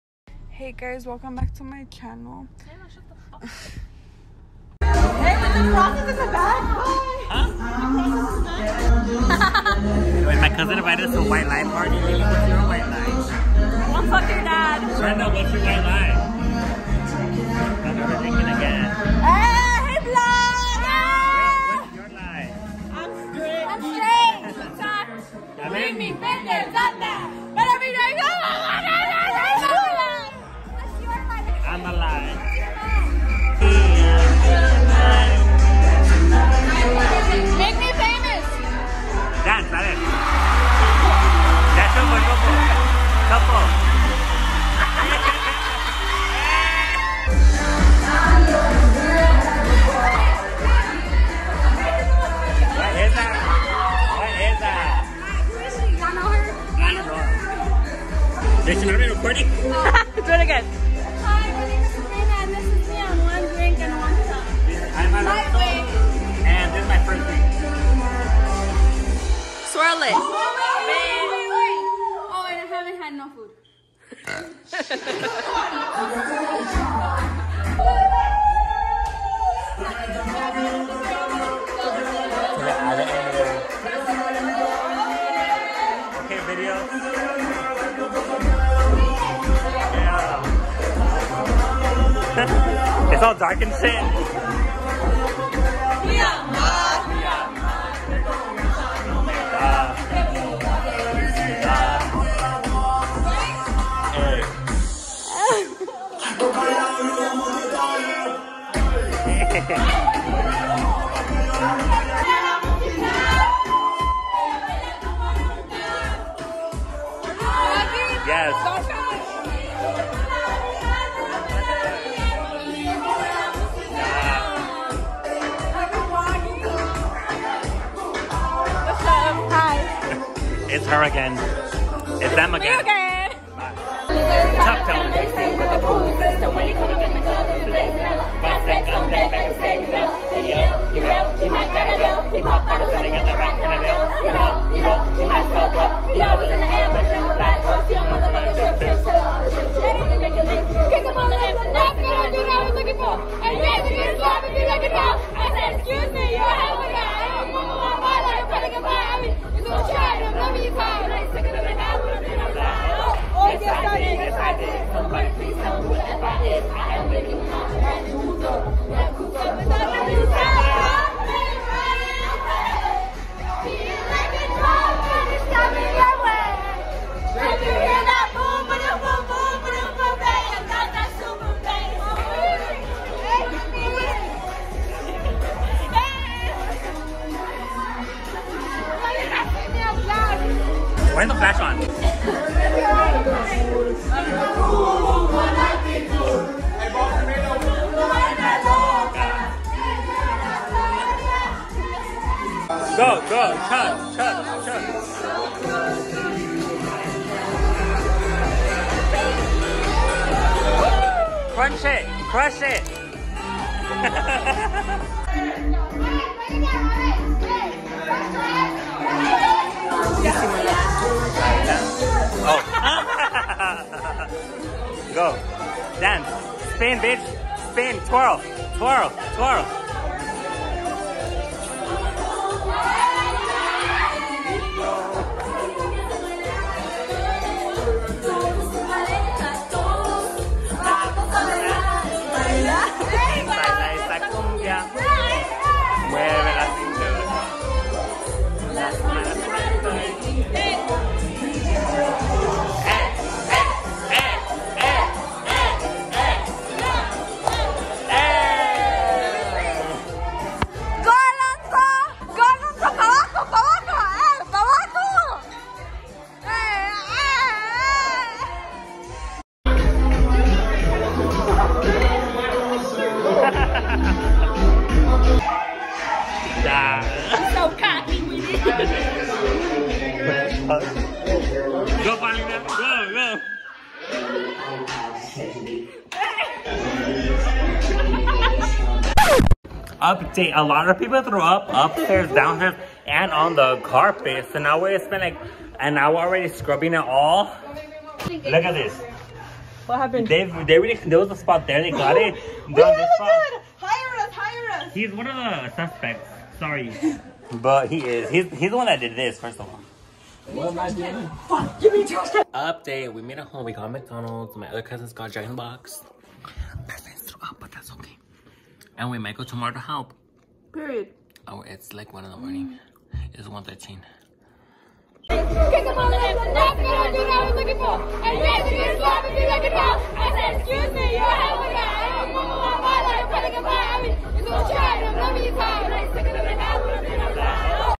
White Lies 🤥 House Party | VLOG
My cousins threw a party while their parents were away 🤫 and our friends threw up 🤢